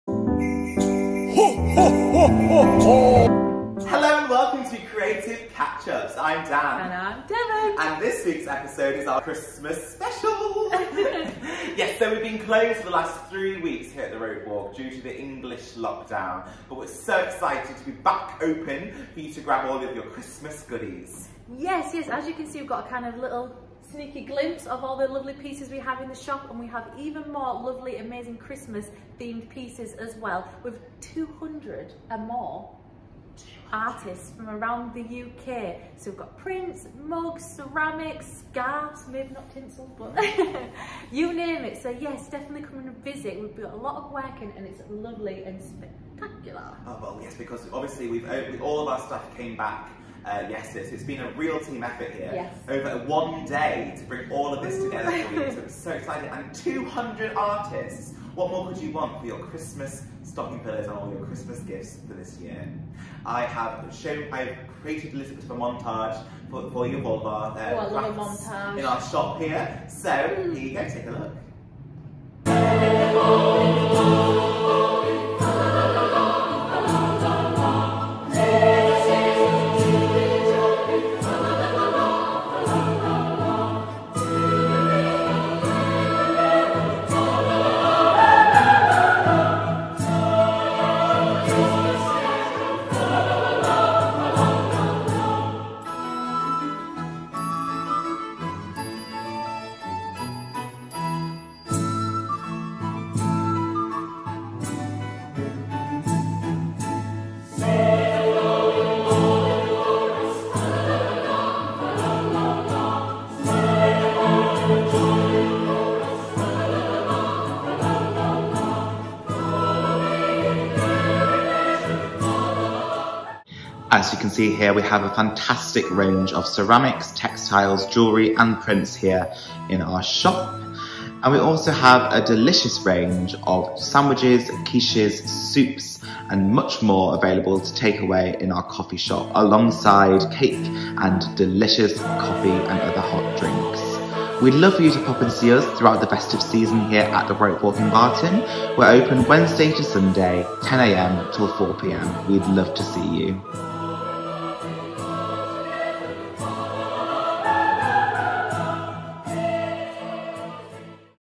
Creative Catchups - Christmas Special
After a short break, Devon and Dan are back with the Christmas Special of Creative Catchups to explore the fantastic work we have featured in our Christmas display!